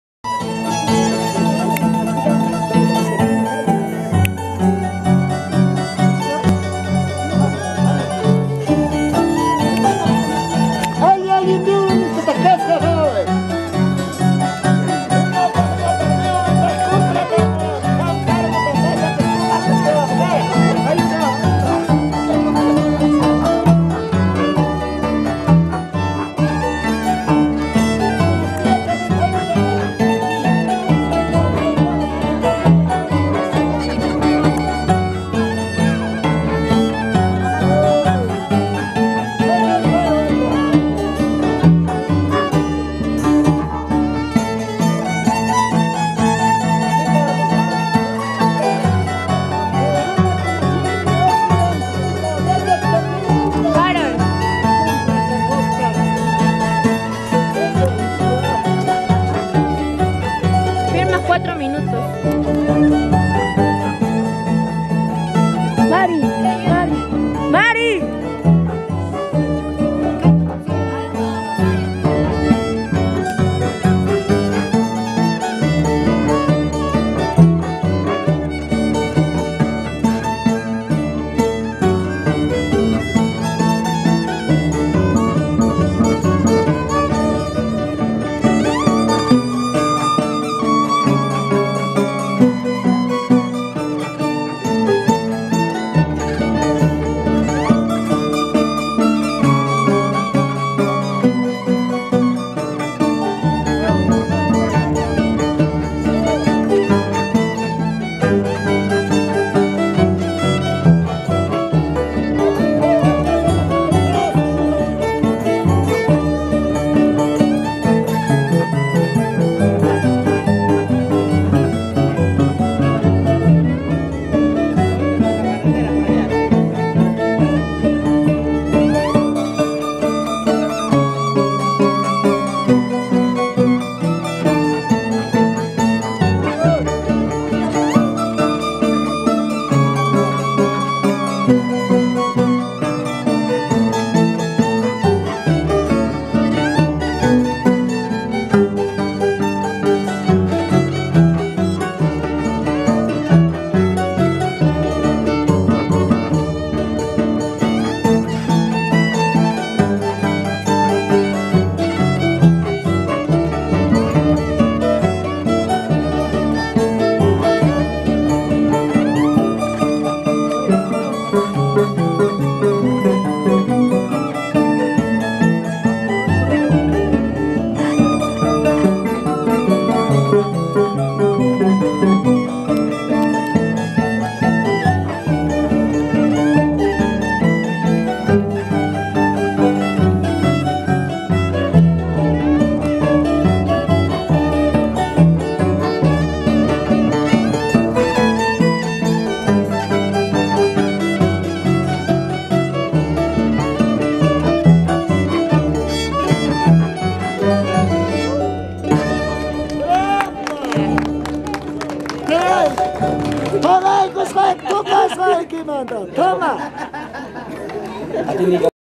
PASCUAL OSORIO tocando marcha en su cargo en la fiesta virgen candelaria de pacapausa 2016
PASCUAL OSORIO TOCANDO MARCHA EN LA FIESTA DE LA VIRGEN CANDELARIA EN PACAPAUZA